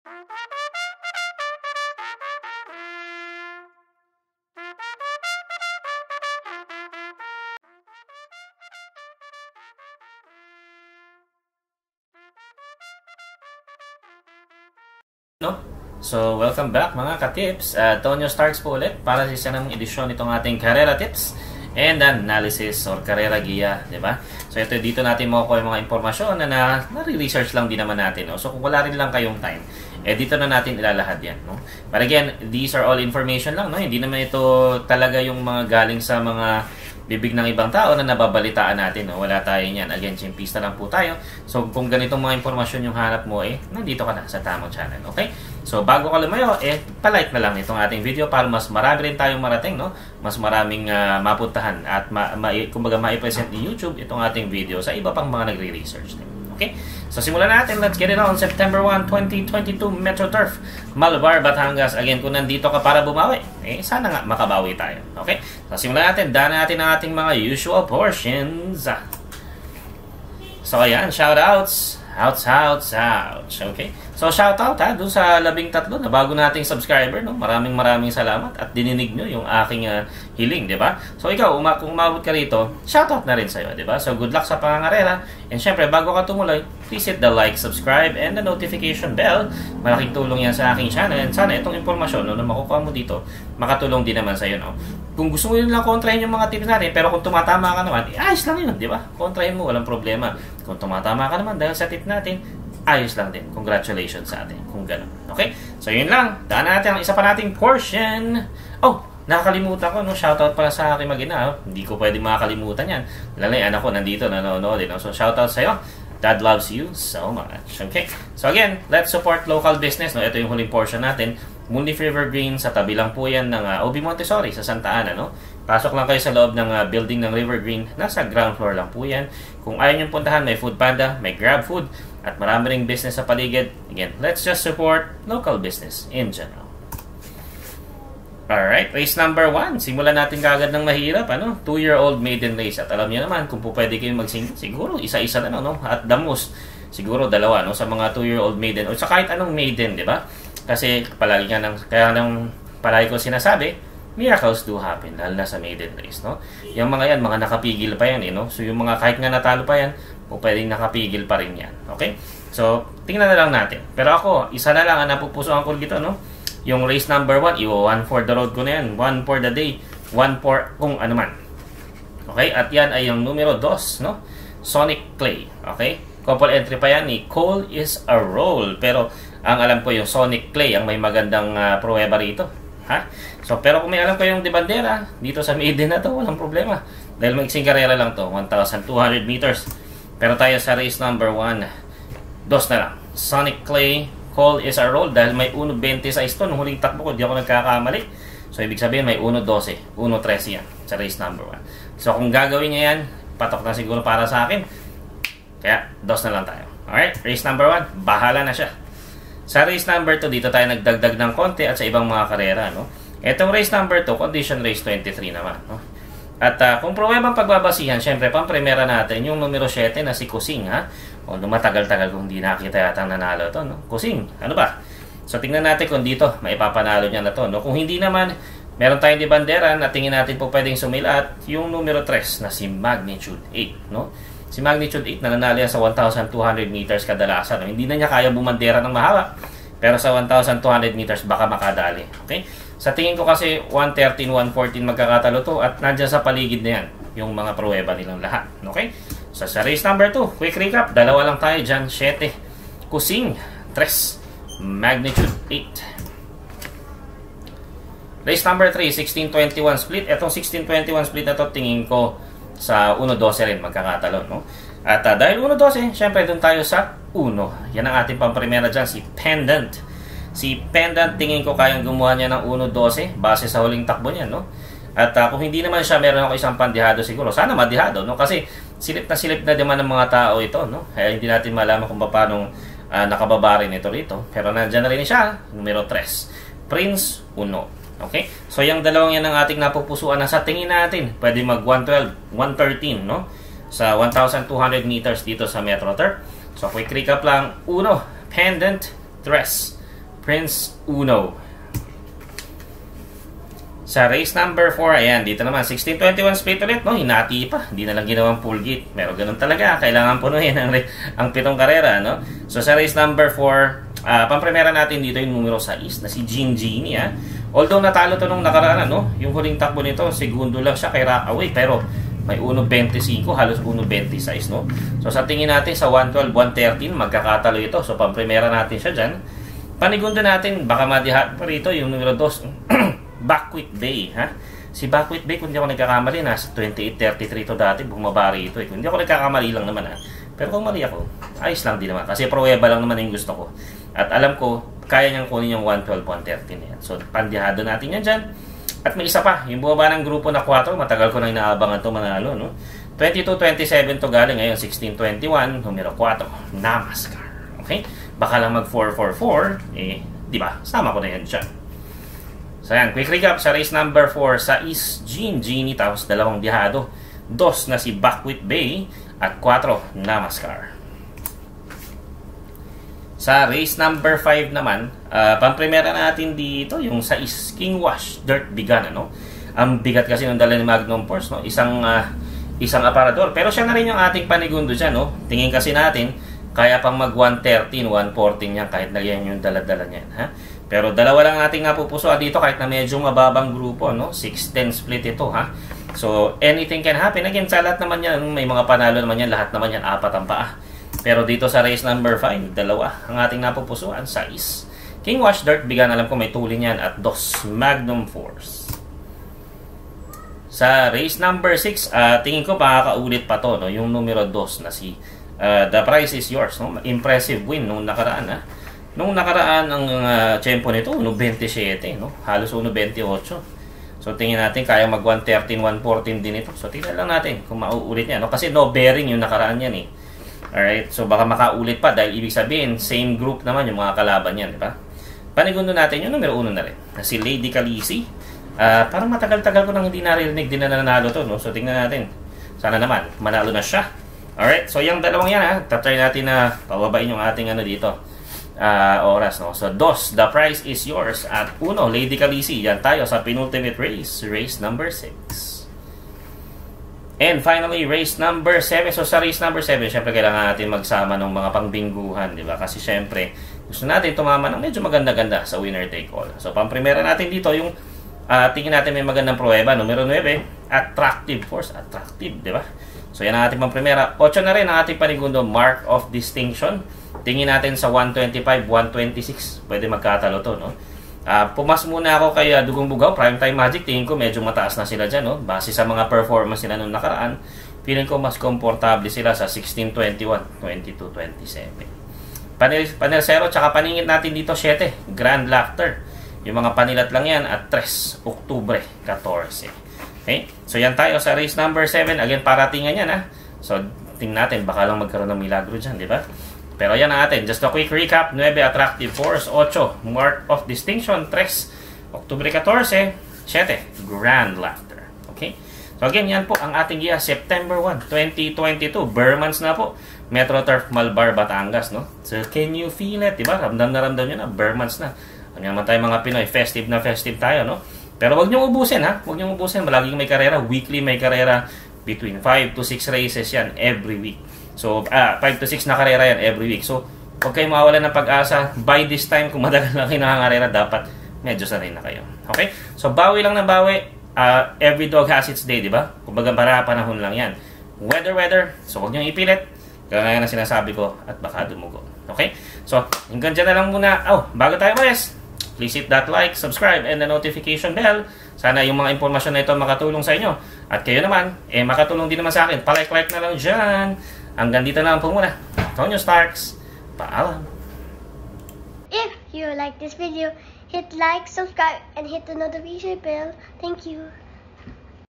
KARERA TIPS and ANALYSIS! - September 1, 2022 - Metroturf - Malvar, Batangas
Giya natin para sa September 1, 2022 - Metroturf - Malvar, Batangas